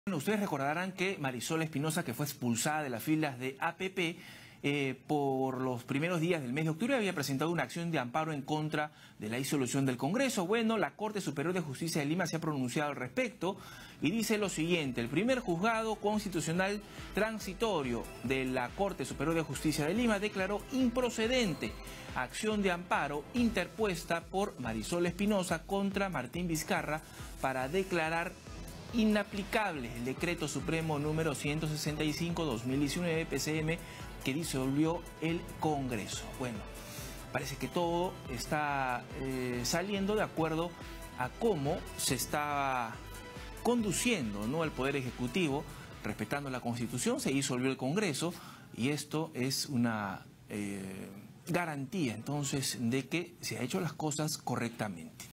PJ declara improcedente acción de amparo contra disolución del Congreso
El Primer Juzgado Constitucional Transitorio de la Corte Superior de Justicia de Lima declaró improcedente la acción de amparo interpuesta por la excongresista Marisol Espinoza para declarar inaplicable el decreto supremo que disolvió el Congreso de la República.